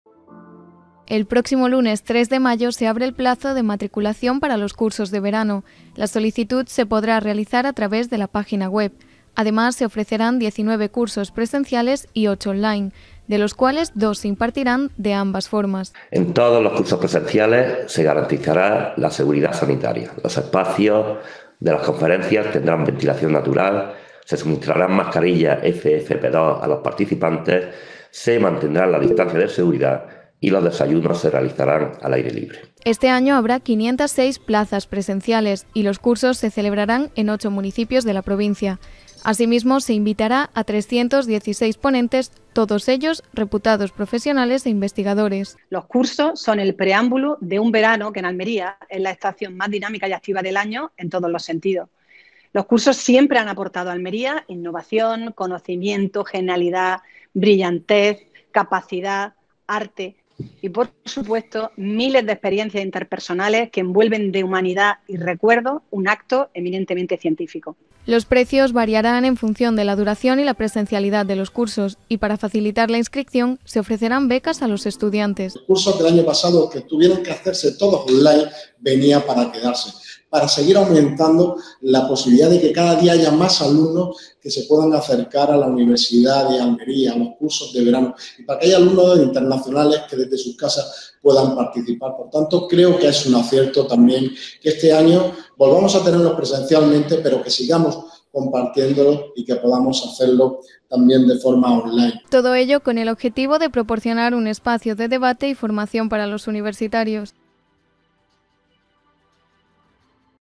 La UAL presenta la XXII edición de los cursos de verano
Una iniciativa que ofertará 27 propuestas formativas tanto en modalidad presencial como online